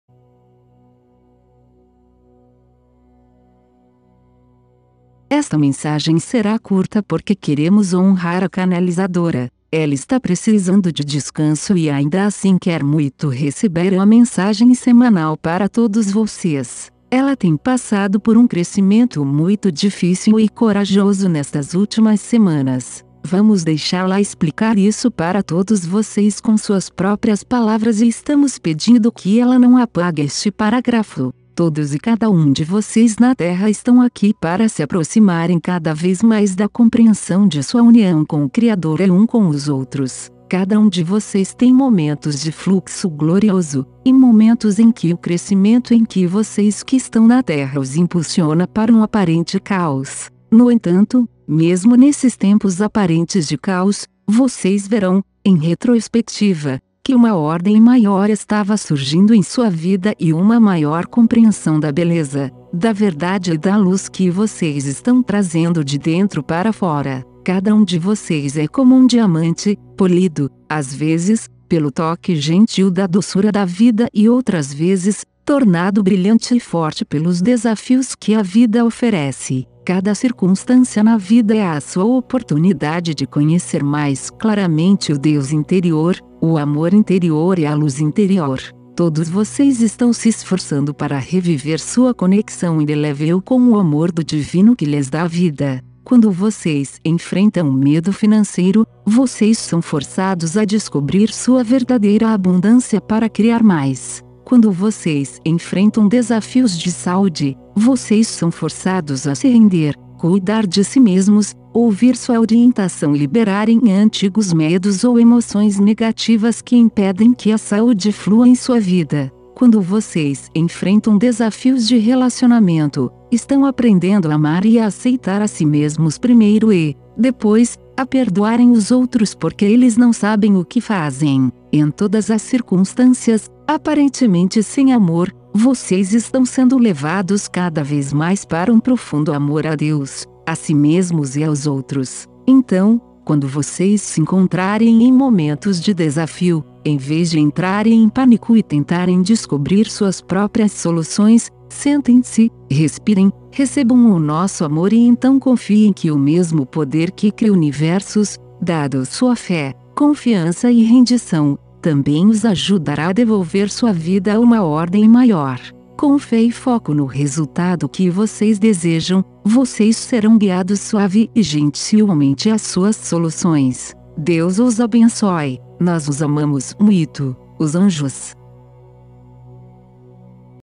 OS ANJOS - "ACOLHENDO A LUZ SOB OS DESAFIOS DA VIDA"
Esta é uma narração da mensagem de   OS ANJOS, postada em texto no blog Sementes das Estrelas.